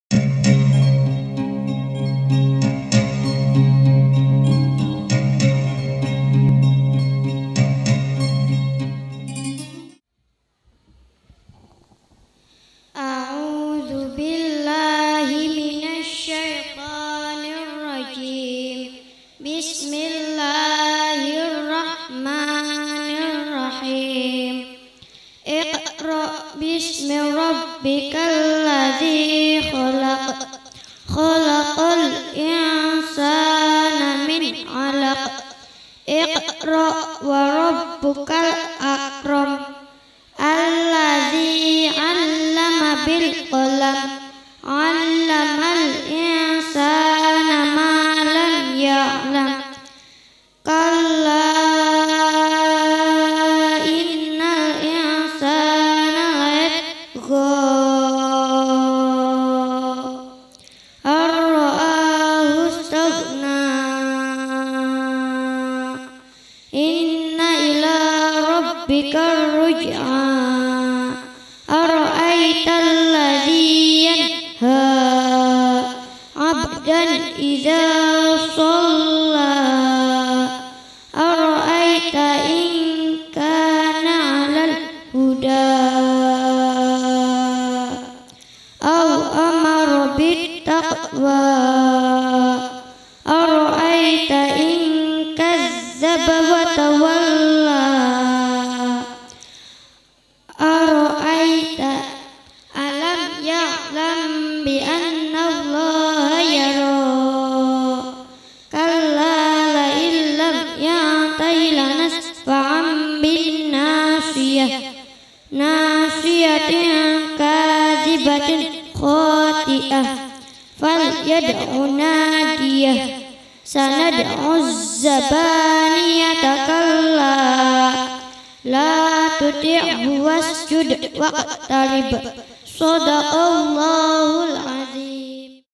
Dokumentasi Khotmil qur'an Juz Amma Mushola Baiturrohim Angkatan 12 Tahun 2009/2010
Dokumentasi Pribadi upload by Fauziyah Multimedia